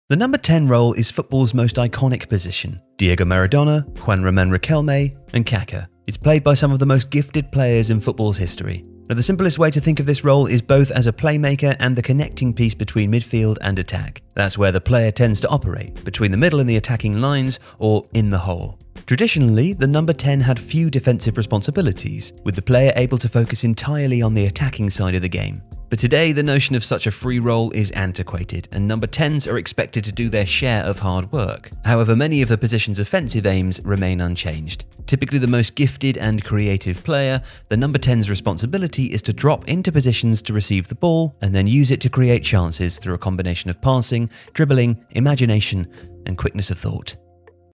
The No.10 Role Explained
Delve into why the Number 10 role is football’s most iconic position. The role brings game-changing impact to the beautiful game. Here it is explained.